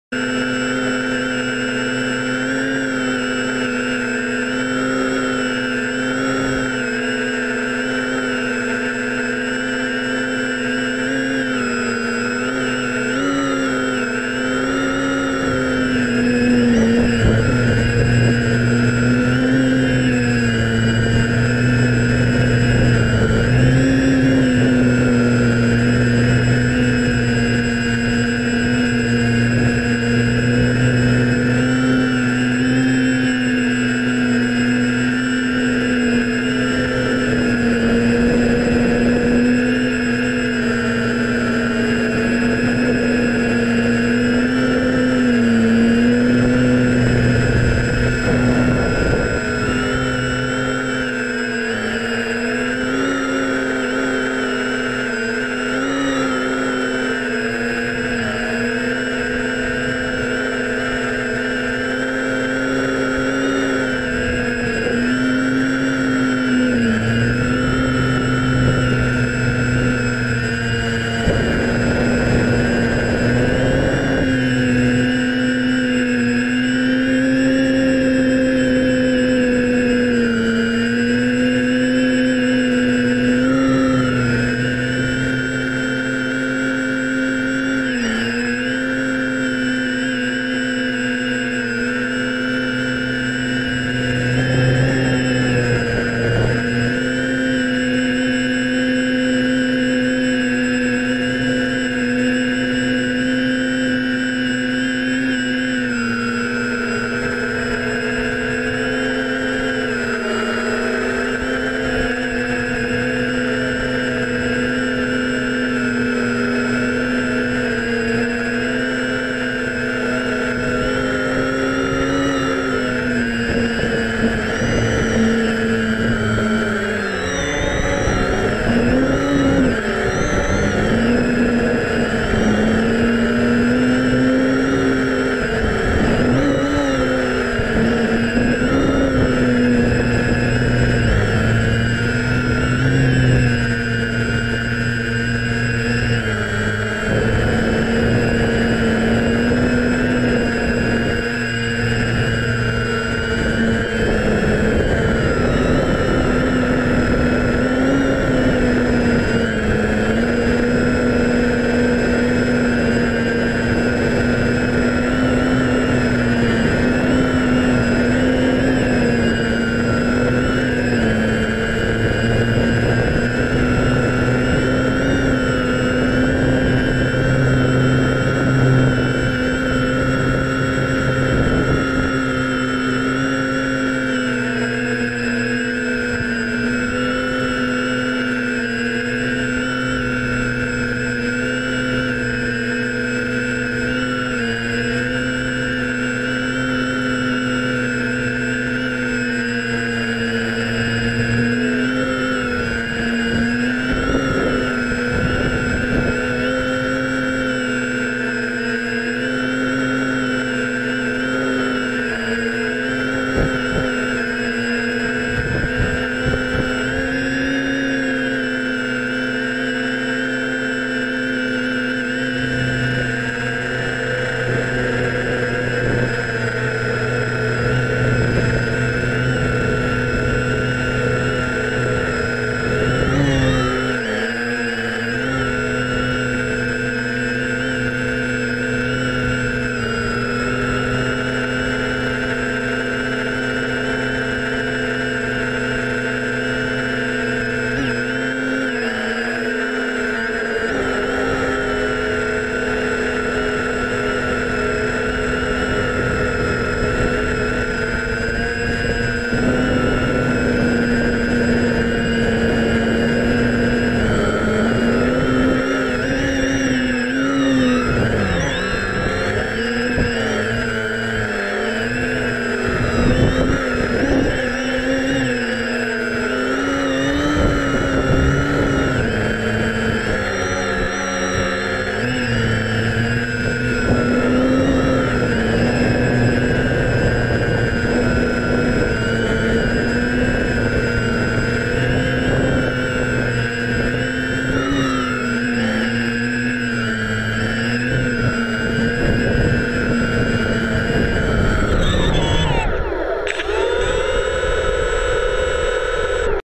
GoPro with my F550 Flamewheel
Just goofing off and testing my Gopro and FPV setup.